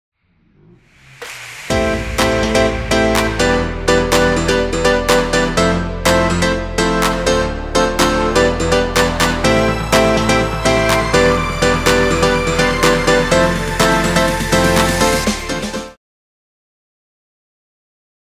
Slo Mo Video Booth Teaser
One of our key banana benders Rainer literally bursting with colour!